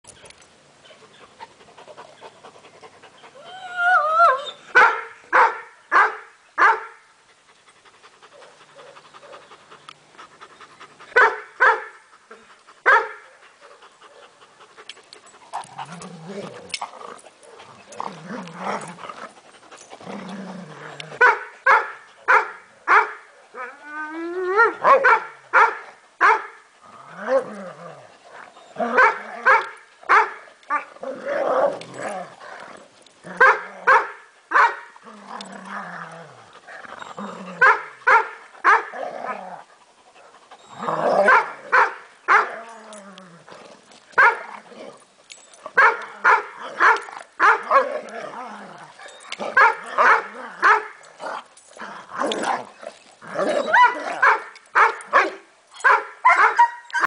Hattie trying to get a tired Lightening to play!  Help Weim rescue of the South in our efforts to save Weimaraners across the southeast by adopting, fostering, or making a financial contribution.  Hattie was adopted from WRS 9 years ago.  LIghtening is a beautiful 1 yr old foster weim who needs an active home!